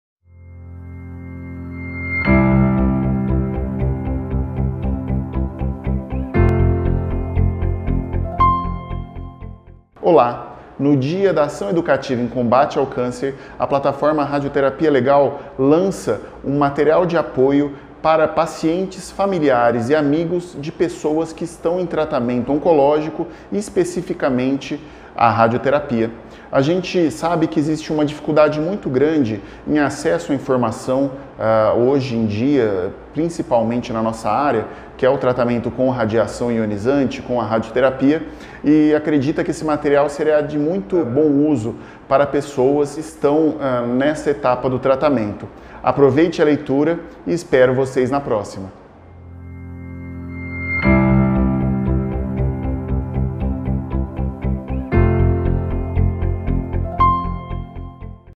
Dia educativo de combate ao câncer
Dia 04 de agosto é o dia nacional da campanha educativa de combate ao câncer. A ideia é que nesse dia diversas ações sejam realizadas a fim de chamar a atenção não apenas para as medidas de prevenção ao câncer, como também orientações sobre os tratamentos existentes para pacientes oncológicos.   Para contribuir com essa ação, criamos um pequeno guia sobre como funciona a radioterapia.